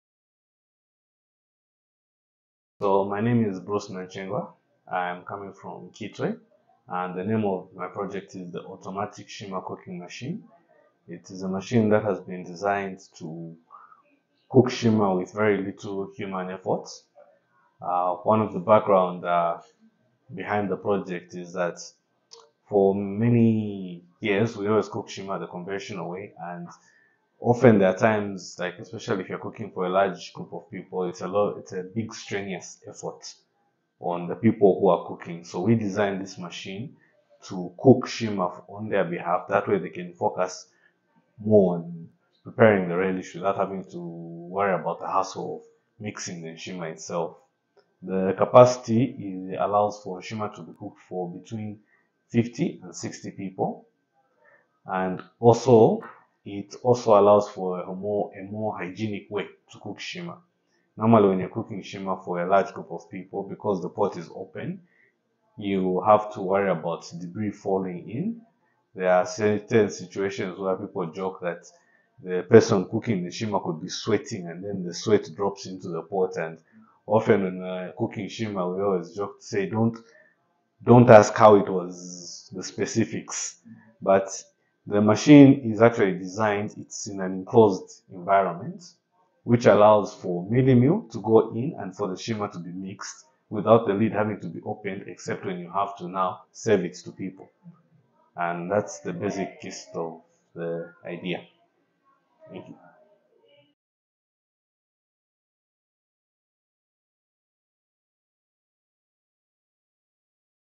ZRDC 2023 National Awards Nominees | Bruce Nanchengwa | Automatic Nshima Cooking Machine
Unveiling the Remarkable 2023 ZRDC National Awards Nominees!

🍲Nominee: Bruce Nanchengwa from Ndola
🥘 Project Name: Automatic Nshima Cooking Machine
🔍 Description: Bruce's innovation is an automatic machine designed for the cooking of nshima, eliminating the need for manual human labor. This groundbreaking invention aims to simplify and automate the process of preparing nshima, a staple food in Zambia, contributing to enhanced convenience and efficiency in the kitchen.

Keep an eye out for more incredible innovators acknowledged at the ZRDC National Awards 2023! 🌐🍲🌟